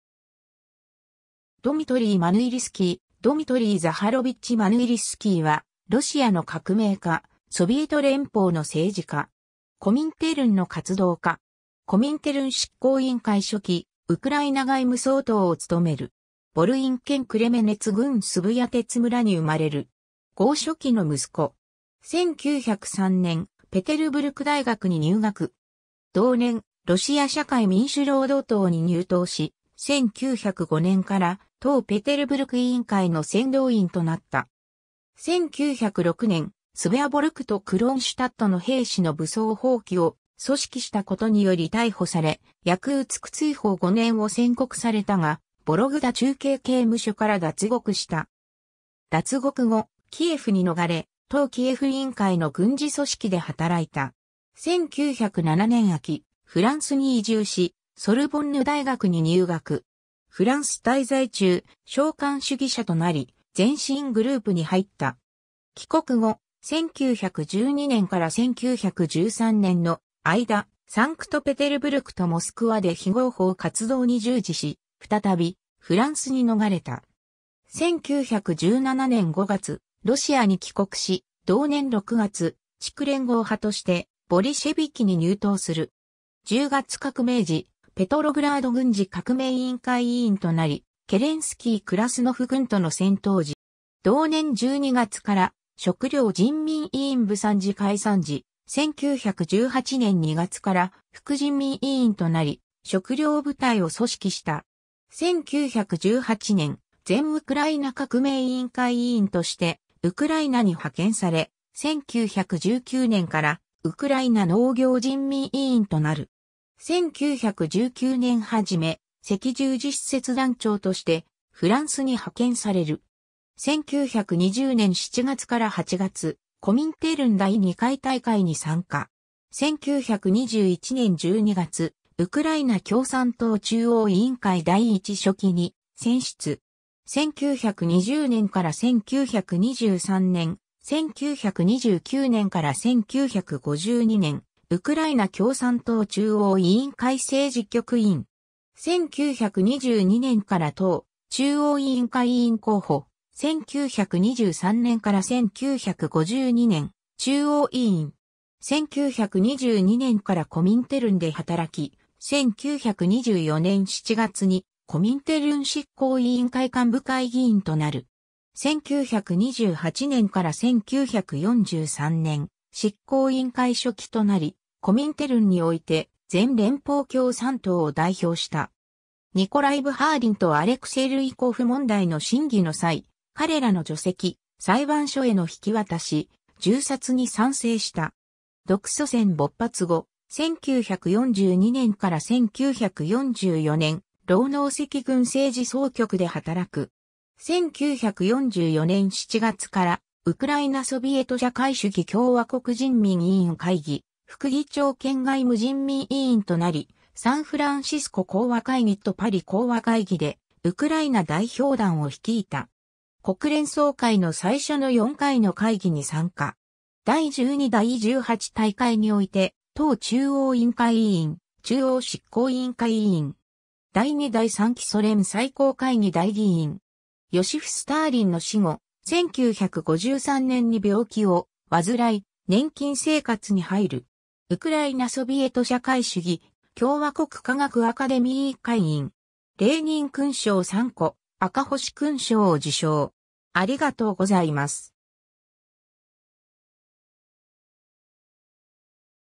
ドミトリー・マヌイリスキー
ドミトリー・マヌイリスキー ドミトリー・ザハロヴィッチ・マヌイリスキー（ロシア語: Дмитрий Захарович Мануильский、1883年9月21日 - 1959年2月22日）は、ロシアの革命家、ソビエト連邦の政治家。
コミンテルンの活動家。
コミンテルン執行委員会書記、ウクライナ外務相等を務める。
 ヴォルィン県クレメネツ郡スヴャテツ村に生まれる。
郷書記の息子。
1903年、ペテルブルク大学に入学（中退）。
同年、ロシア社会民主労働党に入党し、1905年から党ペテルブルク委員会の扇動員となった。
1906年、スヴェアボルクとクロンシュタットの兵士の武装蜂起を組織したことにより逮捕され、ヤクーツク追放5年を宣告されたが、ヴォログダ中継刑務所から脱獄した。
脱獄後、キエフに逃れ、党キエフ委員会の軍事組織で働いた。
 1907年秋、フランスに移住し、ソルボンヌ大学に入学（1911年卒）。
フランス滞在中、召還主義者（ロシア語: отзовист）となり、「前進」（ロシア語: Вперед）グループに入った。
帰国後、1912年～1913年の間、サンクトペテルブルクとモスクワで非合法活動に従事し、再びフランスに逃れた。
 1917年5月、ロシアに帰国し、同年6月、地区連合派（ロシア語: межрайонцы）としてボリシェヴィキに入党する。
十月革命時、ペトログラード軍事革命委員会委員となり、ケレンスキー・クラスノフ軍との戦闘時、クラースノエ・セロー軍事委員として赤衛隊を指揮した。
同年12月から食料人民委員部参事会参事、1918年2月から副人民委員となり、食料部隊を組織した。
 1918年、全ウクライナ革命委員会委員としてウクライナに派遣され、1919年からウクライナ農業人民委員となる。
1919年初め、赤十字使節団長としてフランスに派遣される。
1920年7月～8月、コミンテルン第2回大会に参加。
1921年12月、ウクライナ共産党（ボリシェヴィキ）中央委員会第一書記に選出。
1920年～1923年、1929年～1952年、ウクライナ共産党中央委員会政治局員。
1922年から党中央委員会委員候補、1923年～1952年、中央委員。
 1922年からコミンテルンで働き、1924年7月にコミンテルン執行委員会幹部会議員となる。
1928年～1943年、執行委員会書記となり、コミンテルンにおいて全連邦共産党（ボリシェヴィキ）を代表した。
ニコライ・ブハーリンとアレクセイ・ルイコフ問題の審議の際、彼らの除籍・裁判所への引渡し・銃殺に賛成した。
 独ソ戦勃発後、1942年～1944年、労農赤軍政治総局で働く。
1944年7月からウクライナ・ソビエト社会主義共和国人民委員会議（閣僚会議）副議長兼外務人民委員（外務相）となり、サンフランシスコ講和会議とパリ講和会議でウクライナ代表団を率いた。
国連総会の最初の4回の会議に参加。
 第12～第18大会において、党中央委員会委員、中央執行委員会委員。
第2～第3期ソ連最高会議代議員。
 ヨシフ・スターリンの死後、1953年に病気を患い、年金生活に入る。
 ウクライナ・ソビエト社会主義共和国科学アカデミー会員。
 レーニン勲章3個、赤星勲章を受章。